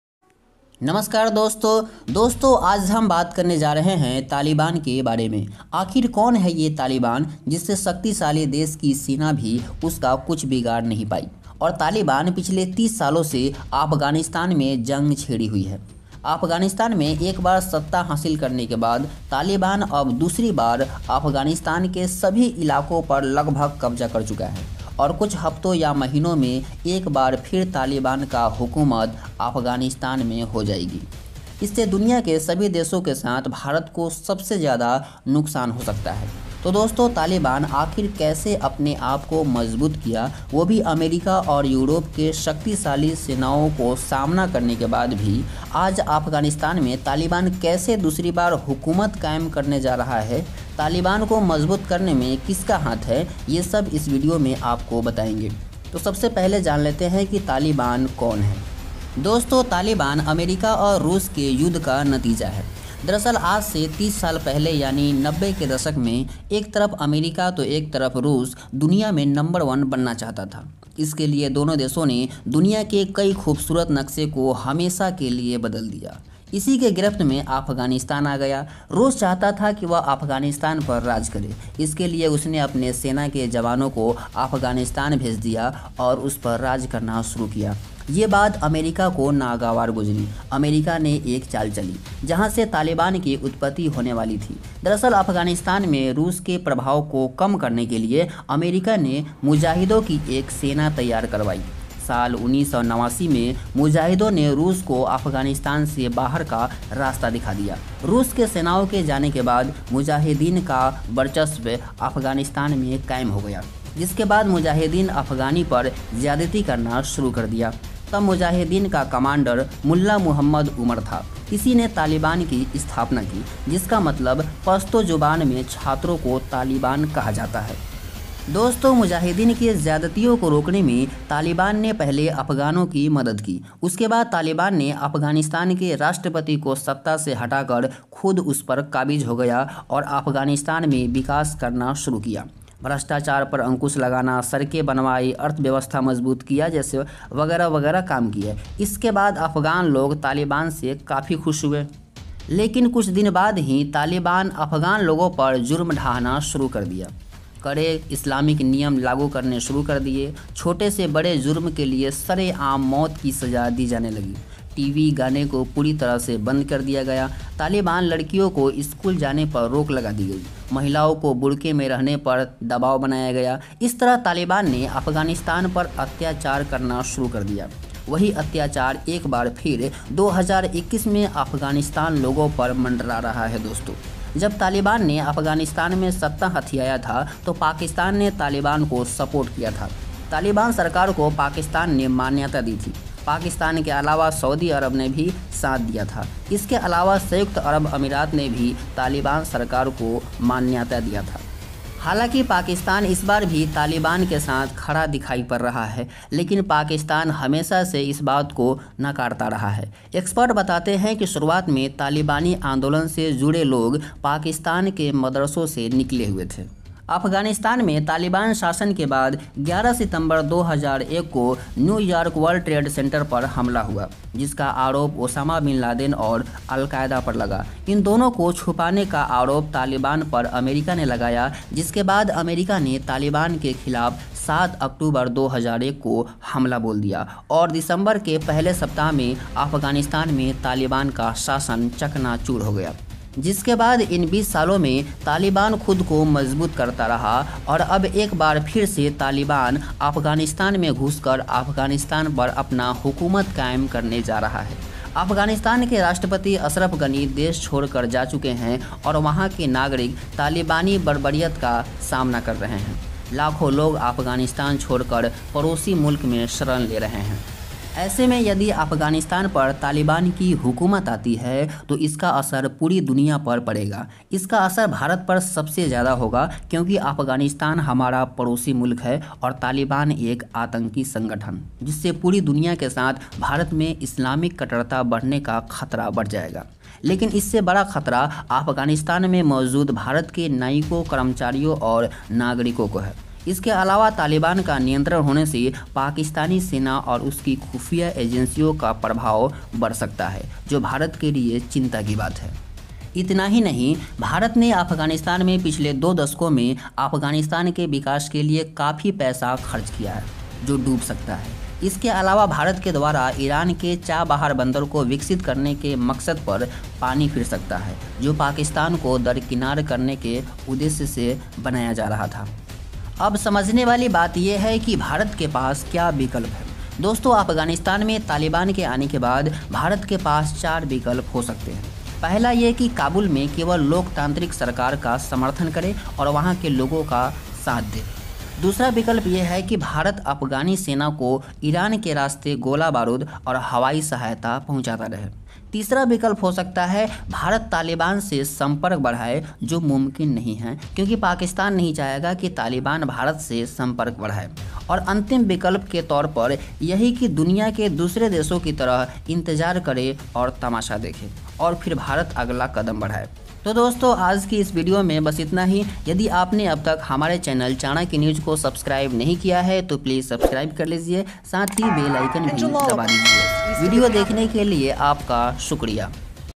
तालिबान हैवान कैसे बना? लड़कियों के साथ करता हैं रेप | जानिए तालिबान का इतिहास | Afghanistan |Taliban
इस वीडियो में तालिबान कौन है? और अफगानिस्तान में तालिबान के दूसरी बार कब्जा करने के मंसूबे के बारे में बात की गई है। साथ ही तालिबान के बढ़ते प्रभाव का भारत और दुनिया पर क्या असर पड़ेगा, इसके बारे में बताई गई है। और तालिबान की क्रूरता के बारे में बताई गई है। जरूर देखें।